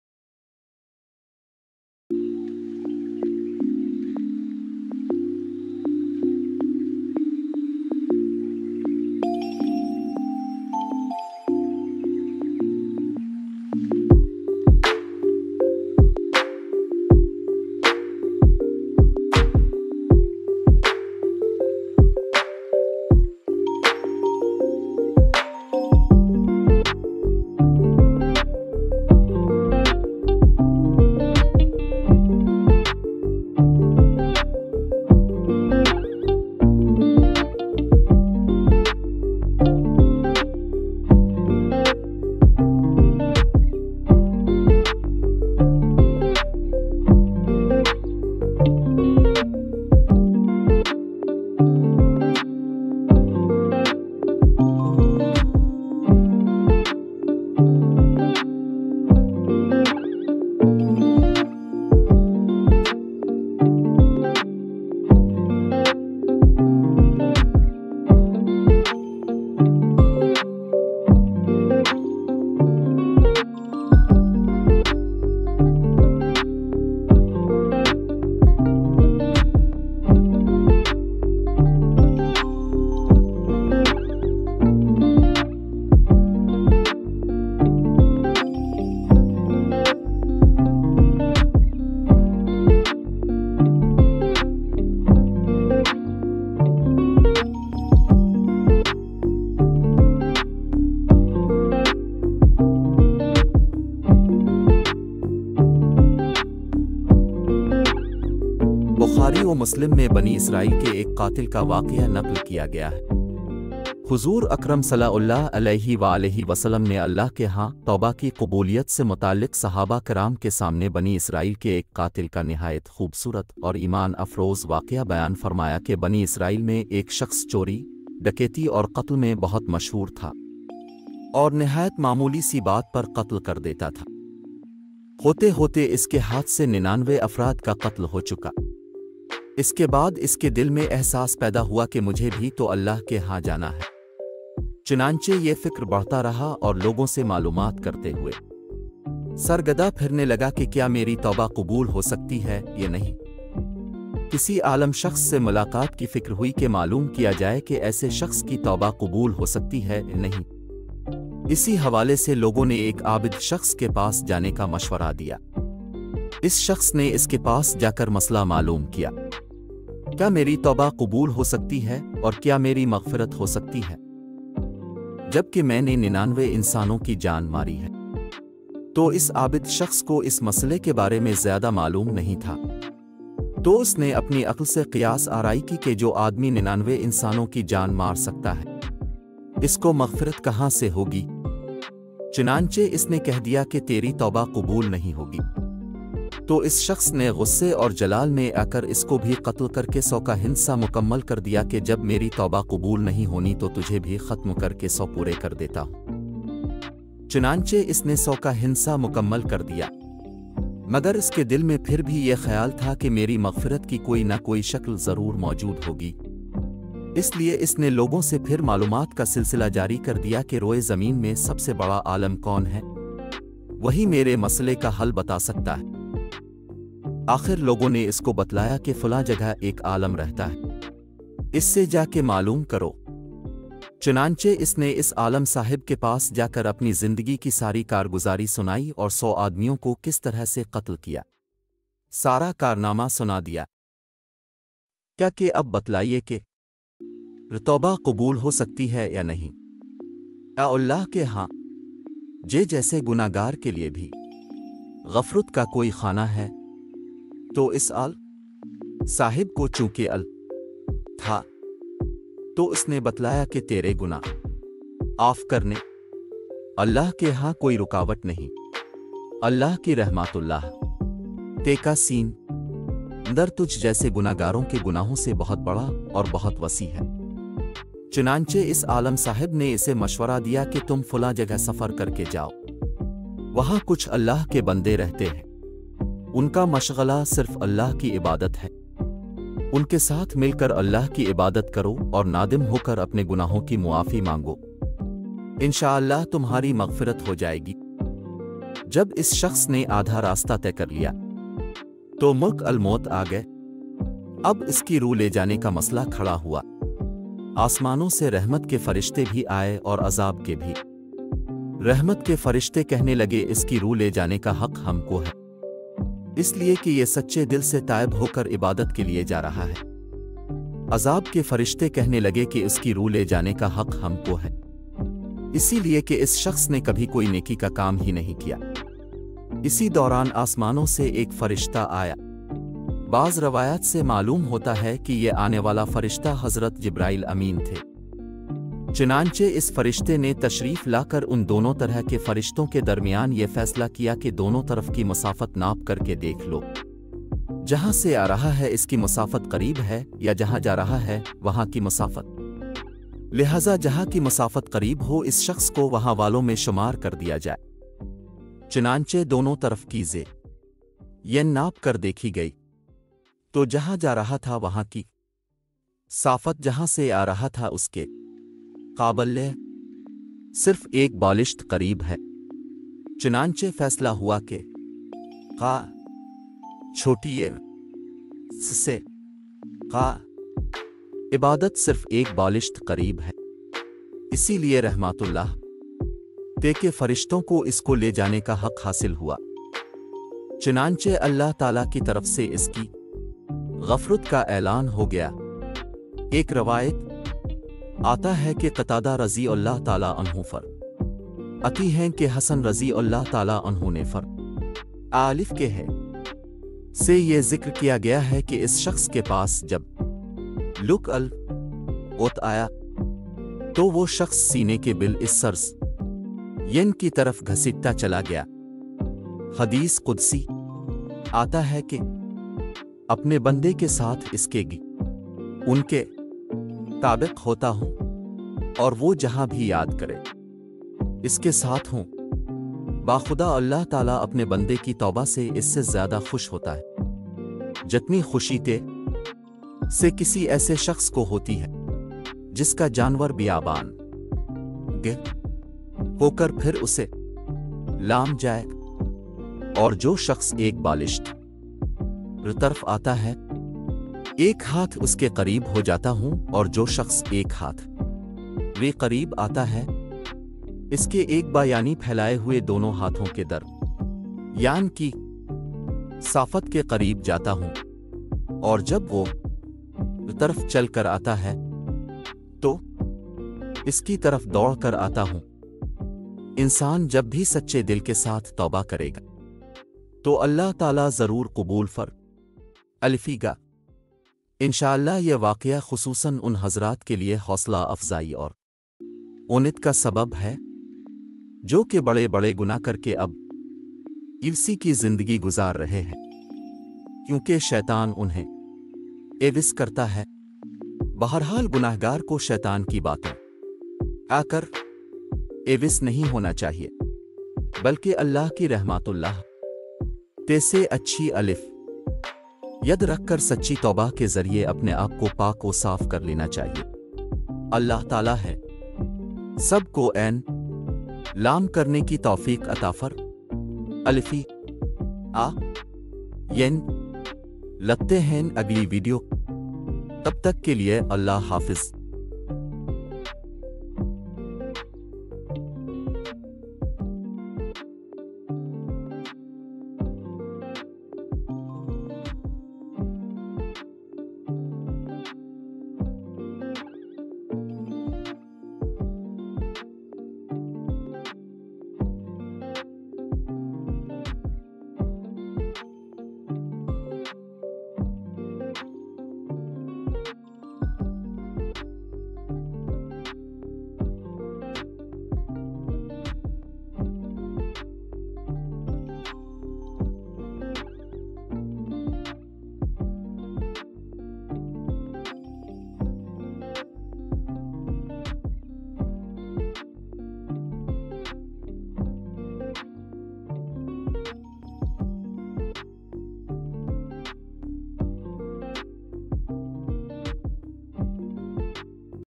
Bani Israel ka waqia | bani israel history in urdu | Bani israel kon thay | BY Faheem Chhipa
بخاری و مسلم میں بنی اسرائیل کے ایک قاتل کا واقعہ نقل کیا گیا ہے۔ حضور اکرم صلی اللہ علیہ وآلہ وسلم نے اللہ کے ہاں توبہ کی قبولیت سے متعلق صحابہ کرام کے سامنے بنی اسرائیل کے ایک قاتل کا نہایت خوبصورت اور ایمان افروز واقعہ بیان فرمایا کہ بنی اسرائیل میں ایک شخص چوری، ڈکیتی اور قتل میں بہت مشہور تھا۔

اور نہایت معمولی سی بات پر قتل کر دیتا تھا۔ ہوتے ہوتے اس کے ہاتھ سے ننانوے افراد کا قتل ہو چکا۔ اس کے بعد اس کے دل میں احساس پیدا ہوا کہ مجھے بھی تو اللہ کے ہاں جانا ہے۔ چنانچہ یہ فکر بڑھتا رہا اور لوگوں سے معلومات کرتے ہوئے۔ سرگدا پھرنے لگا کہ کیا میری توبہ قبول ہو سکتی ہے یا نہیں؟

کسی عالم شخص سے ملاقات کی فکر ہوئی کہ معلوم کیا جائے کہ ایسے شخص کی توبہ قبول ہو سکتی ہے یا نہیں؟ اسی حوالے سے لوگوں نے ایک عابد شخص کے پاس جانے کا مشورہ دیا۔ اس شخص نے اس کے پاس جا کر مسئلہ معلوم کیا۔ کیا میری توبہ قبول ہو سکتی ہے اور کیا میری مغفرت ہو سکتی ہے؟

جب کہ میں نے ننانوے انسانوں کی جان ماری ہے۔ تو اس عابد شخص کو اس مسئلے کے بارے میں زیادہ معلوم نہیں تھا۔ تو اس نے اپنی عقل سے قیاس آرائی کی کہ جو آدمی ننانوے انسانوں کی جان مار سکتا ہے۔ اس کو مغفرت کہاں سے ہو گی۔

چنانچہ اس نے کہہ دیا کہ تیری توبہ قبول نہیں ہوگی۔ تو اس شخص نے غصے اور جلال میں آ کر اس کو بھی قتل کر کے سو کا ہندسہ مکمل کر دیا کہ جب میری توبہ قبول نہیں ہونی تو تجھے بھی ختم کر کے سو پورے کر دیتا ہوں۔

چنانچہ اس نے سو کا ہندسہ مکمل کر دیا۔ مگر اس کے دل میں پھر بھی یہ خیال تھا کہ میری مغفرت کی کوئی نہ کوئی شکل ضرور موجود ہو گی۔ اس لیے اس نے لوگوں سے پھر معلومات کا سلسلہ جاری کر دیا کہ روئے زمین میں سب سے بڑا عالم کون ہے؟ وہی میرے مسئلے کا حل بتا سکتا ہے۔

آخر لوگوں نے اس کو بتلایا کہ فلاں جگہ ایک عالم رہتا ہے۔ اس سے جا کے معلوم کرو۔ چنانچہ اس نے اس عالم صاحب کے پاس جا کر اپنی زندگی کی ساری کارگزاری سنائی اور سو آدمیوں کو کس طرح سے قتل کیا۔ سارا کارنامہ سنا دیا۔

معلوم کیا کہ اب بتلائیے کہ میری توبہ قبول ہو سکتی ہے یا نہیں؟ کیا اللہ کے ہاں مجھ جیسے گناہ گار کے لیے بھی مغفرت کا کوئی خانہ ہے؟ تو اس عالم صاحب کو چونکہ علم تھا۔ تو اس نے بتلایا کہ تیرے گناہ معاف کرنے میں اللہ کے ہاں کوئی رکاوٹ نہیں۔ اللہ کی رحمت کا سمندر تجھ جیسے گناہ گاروں کے گناہوں سے بہت بڑا اور بہت وسیع ہے۔

چنانچہ اس عالم صاحب نے اسے مشورہ دیا کہ تم فلاں جگہ سفر کر کے جاؤ۔ وہاں کچھ اللہ کے بندے رہتے ہیں۔ ان کا مشغلہ صرف اللہ کی عبادت ہے۔ ان کے ساتھ مل کر اللہ کی عبادت کرو اور نادم ہو کر اپنے گناہوں کی معافی مانگو۔ انشاءاللہ تمہاری مغفرت ہو جائے گی۔

جب اس شخص نے آدھا راستہ طے کر لیا۔ تو ملک الموت آگئے۔ اب اس کی روح لے جانے کا مسئلہ کھڑا ہوا۔ آسمانوں سے رحمت کے فرشتے بھی آئے اور عذاب کے بھی۔ رحمت کے فرشتے کہنے لگے اس کی روح لے جانے کا حق ہم کو ہے۔ اس لیے کہ یہ سچے دل سے تائب ہو کر عبادت کے لیے جا رہا ہے۔

عذاب کے فرشتے کہنے لگے کہ اس کی روح لے جانے کا حق ہم کو ہے۔ اسی لیے کہ اس شخص نے کبھی کوئی نیکی کا کام ہی نہیں کیا۔ اسی دوران آسمانوں سے ایک فرشتہ آیا۔ بعض روایات سے معلوم ہوتا ہے کہ یہ آنے والا فرشتہ حضرت جبرائیل امین تھے۔

چنانچہ اس فرشتے نے تشریف لا کر ان دونوں طرح کے فرشتوں کے درمیان یہ فیصلہ کیا کہ دونوں طرف کی مسافت ناپ کر کے دیکھ لو۔ جہاں سے آ رہا ہے اس کی مسافت قریب ہے یا جہاں جا رہا ہے وہاں کی مسافت۔ لہذا جہاں کی مسافت قریب ہو اس شخص کو وہاں والوں میں شمار کر دیا جائے۔

چنانچہ دونوں طرف کی زمین ناپ کر دیکھی گئی۔ تو جہاں جا رہا تھا وہاں کی مسافت جہاں سے آ رہا تھا اس کے مقابلے میں صرف ایک بالشت قریب ہے۔ چنانچہ فیصلہ ہوا کہ مقامی مصیحت سے مقام عبادت صرف ایک بالشت قریب ہے۔ اسی لیے رحمت کے فرشتوں کو اس کو لے جانے کا حق حاصل ہوا۔

چنانچہ اللہ تعالیٰ کی طرف سے اس کی مغفرت کا اعلان ہو گیا۔ ایک روایت میں آتا ہے کہ قتادہ رضی اللہ تعالیٰ عنہ فرماتے ہیں کہ حسن رضی اللہ تعالیٰ عنہ نے فرمایا کہ ہم سے یہ ذکر کیا گیا ہے کہ اس شخص کے پاس جب ملک الموت آیا۔ تو وہ شخص سینے کے بل اس سرزمین کی طرف گھسیٹتا چلا گیا۔
                                                                                                                 -:  حدیث قدسی
حدیث قدسی میں آتا ہے کہ میں اپنے بندے کے ساتھ اس کے گمان کے مطابق ہوتا ہوں۔ اور وہ جہاں بھی یاد کرے میں اس کے ساتھ ہوں۔ باخدا اللہ تعالی اپنے بندے کی توبہ سے اس سے زیادہ خوش ہوتا ہے۔ جتنی خوشی تم میں سے کسی ایسے شخص کو ہوتی ہے۔ جس کا جانور بیابان میں گم ہو کر پھر اسے مل جائے۔

اور جو شخص ایک بالشت میری طرف آتا ہے۔ میں ایک ہاتھ اس کے قریب ہو جاتا ہوں اور جو شخص ایک ہاتھ میرے قریب آتا ہے۔ میں اس کے ایک باع یعنی پھیلائے ہوئے دونوں ہاتھوں کے درمیان کی مسافت کے قریب جاتا ہوں۔ اور جب وہ میری طرف چل کر آتا ہے۔ تو میں اس کی طرف دوڑ کر آتا ہوں۔

BANI ISRAEL | Bani israel ka waqia | BANI ISRARL KON THAY | Molana tariq jameel | By Faheem Chhipa